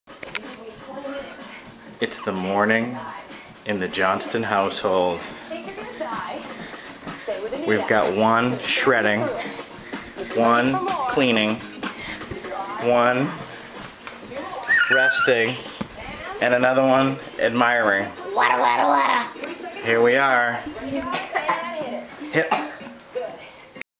The AM Shred Machine
Early morning fitness as Pam shreds with Julian's DVD.